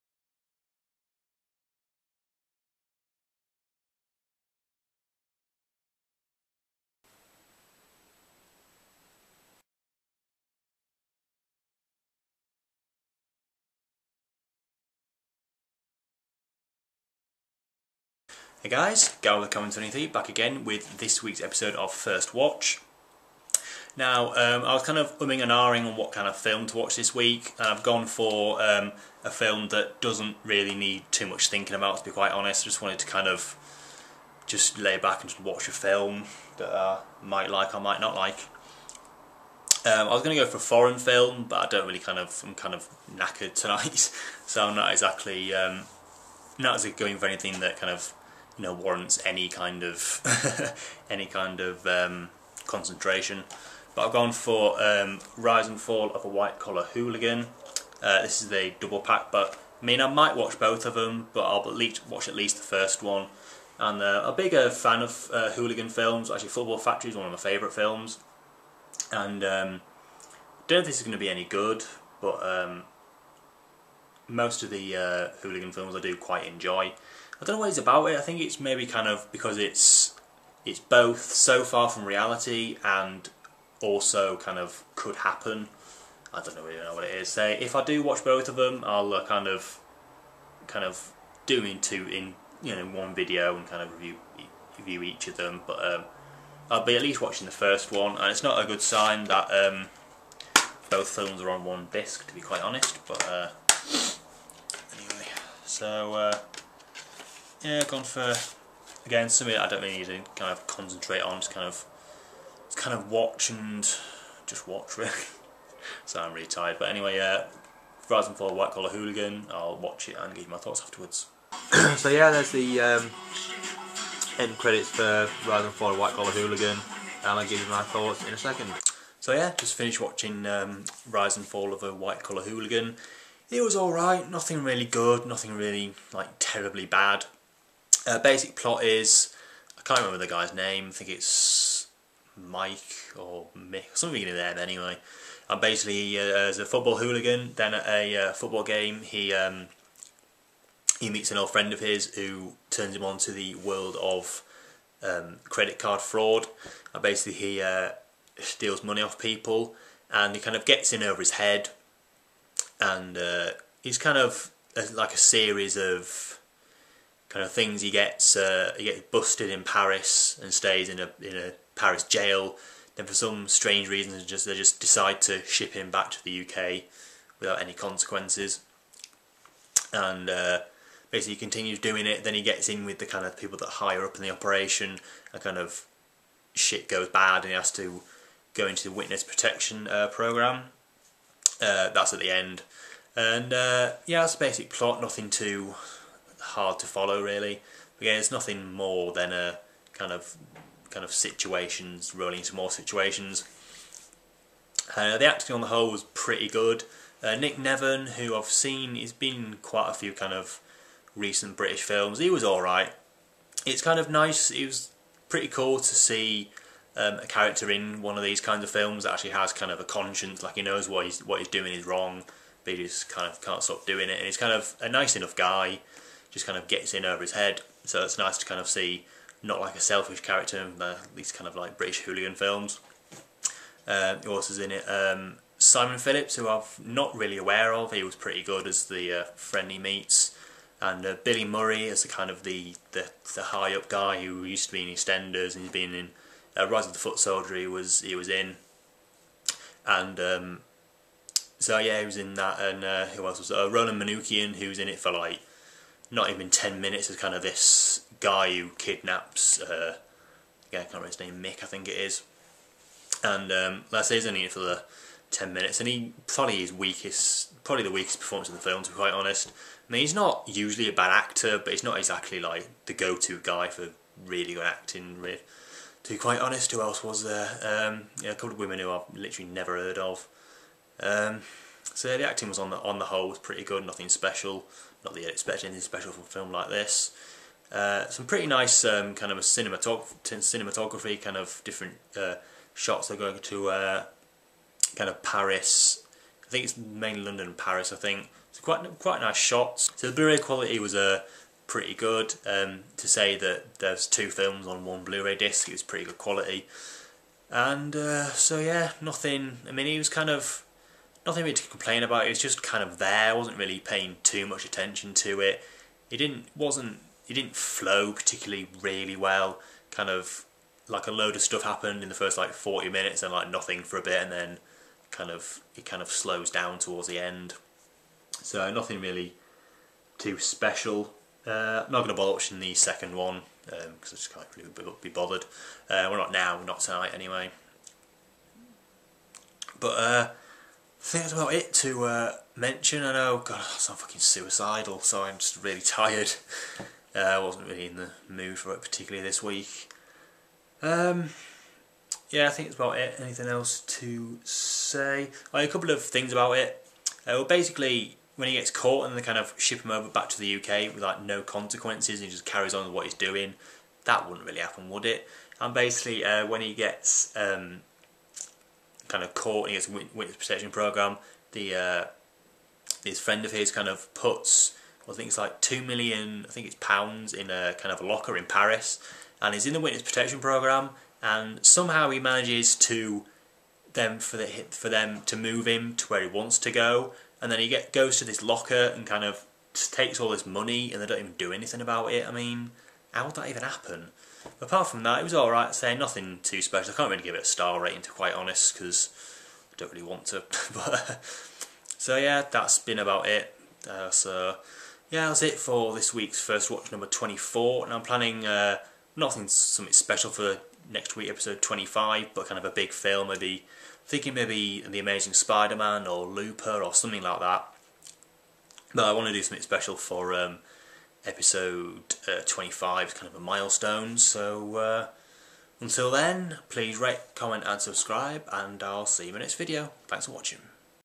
First Watch #24 Rise and Fall of a White Collar Hooligan (2012)
In this video I will be watching and giving my thoughts on the british film The Rise and Fall of a White Collar Hooligan. Directed by Paul Tanter and Starring Nick Nevern, Simon Phillips, Roland Manookian, Ricci Hartnett and Billy Murray.

Add me on Facebook-Peter Fletcher